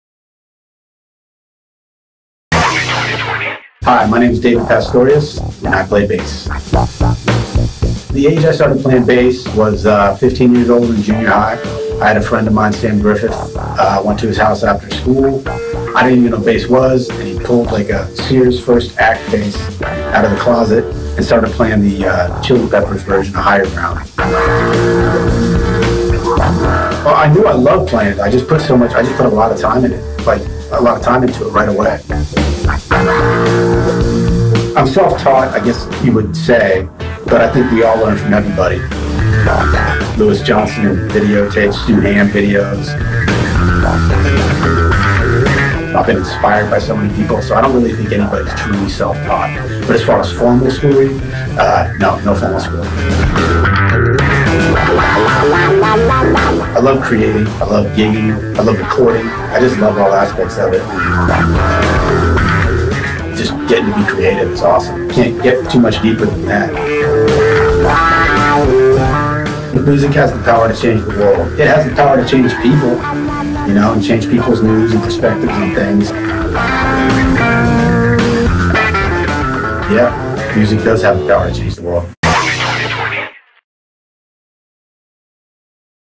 WHO IS DAVID PASTORIUS - MORLEY 20/20
Florida Bass Player David Pastorius Morley Bass Players United in Wah short inspiring Interview. In the spring of 2020 Morley presents a special project featuring members of the international Bass community such as David Pastorius, Alex Lofoco, Cody Wright, Chris Van Der Walt,
Quintin Berry, Brandino, Michel Laredo aka Momo